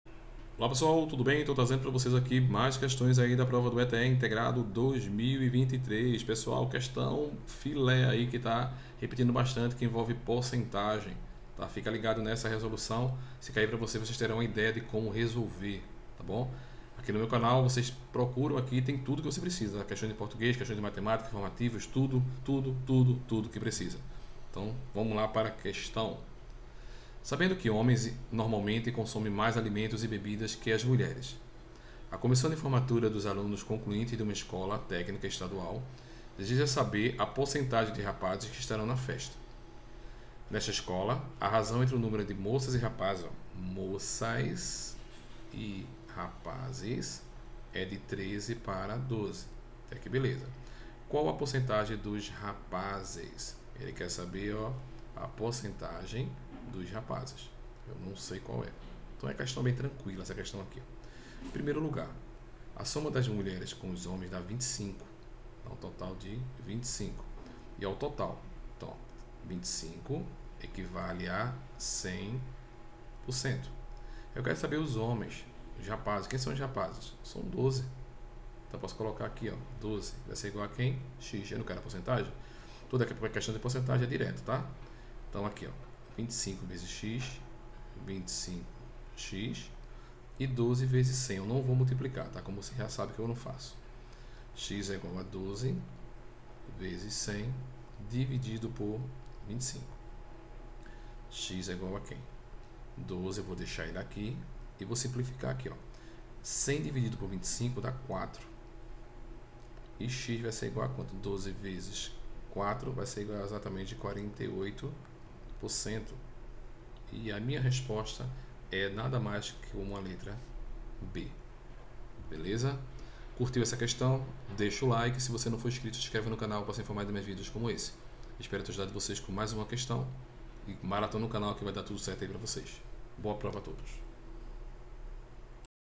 QUESTAO DOS HOMENS QUE CONSOMEM MAIS ALIMENTOS QUE AS MULHERES DO ETE 2023 8º DIA
ESTE CANAL TEM O INTUITO DE AJUDAR VOCE A SE DAR BEM EM MATEMÁTICA BÁSICA, PARA ETE PE, IFPE, SSA E ENEM.